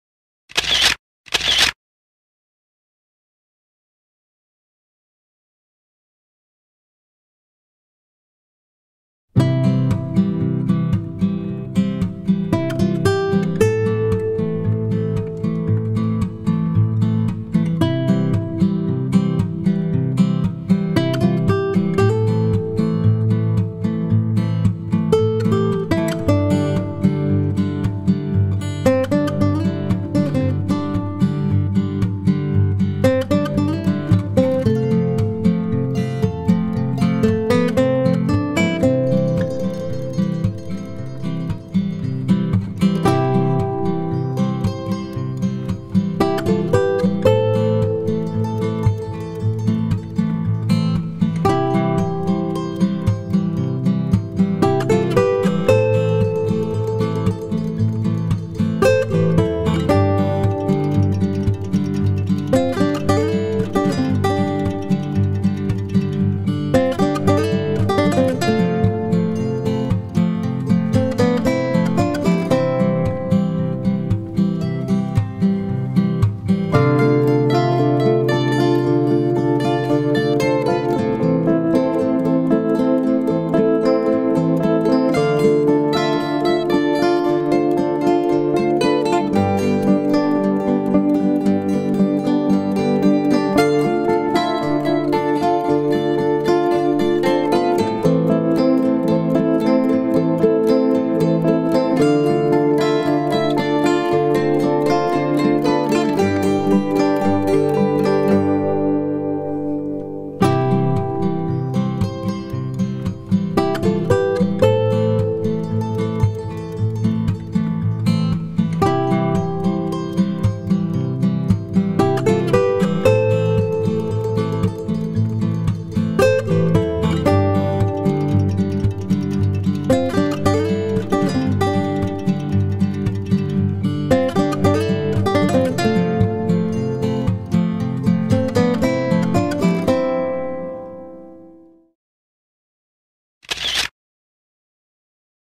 France. Le Vieux Village De Biot.
Biot est située entre Cannes et Nice, elle est limitrophe de la ville d'Antibes. Biot compose, avec Antibes, Mougins, Valbonne et Vallauris la technopole de Sophia-Antipolis.